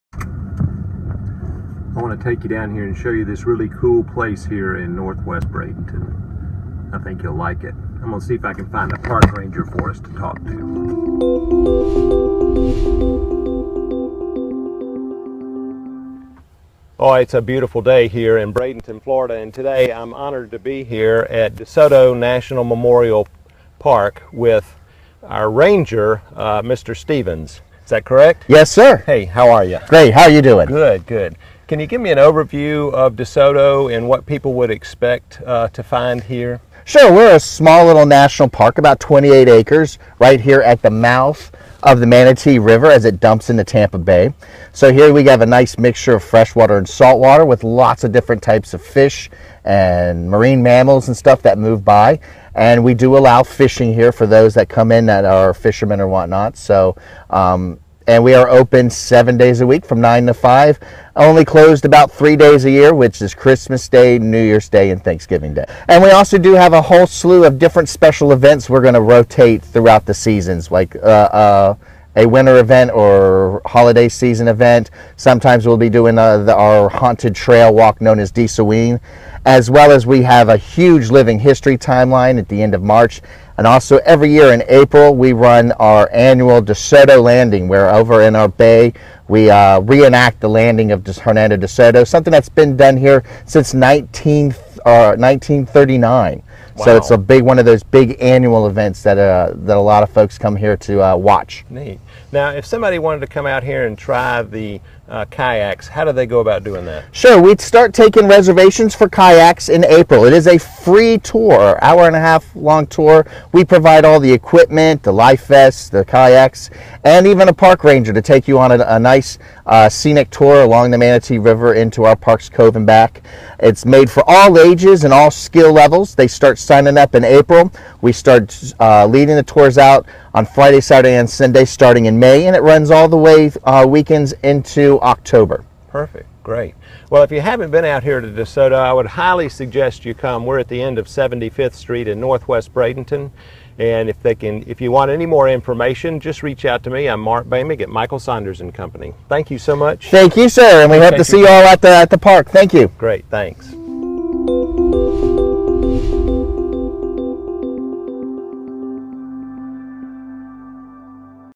DeSoto National Memorial Park, Bradenton FL | Mark Boehmig
This is a great place to visit while you are in Bradenton.  Beautiful walking trails, areas to fish, and free kayak tours on the weekends.  An inside welcome center, with a movie about DeSoto and his travels to this part of Florida.